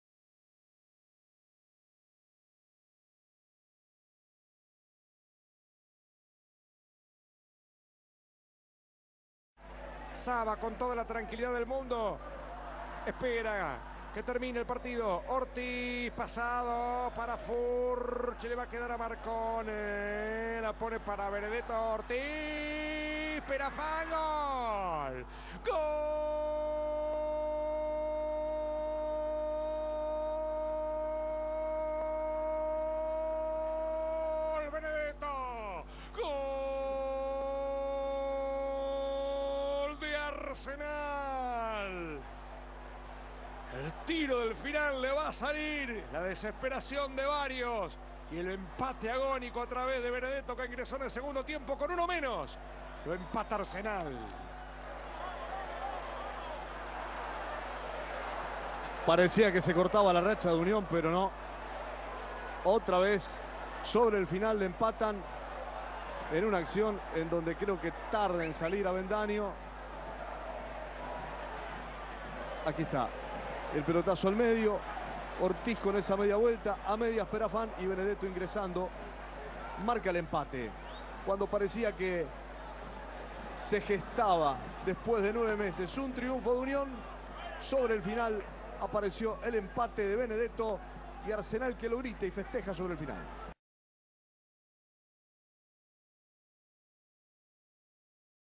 Torneo Final 2013 :: Fecha 1 :: Unión 1 - Arsenal 1 :: Gol
Gol de Darío Benedetto.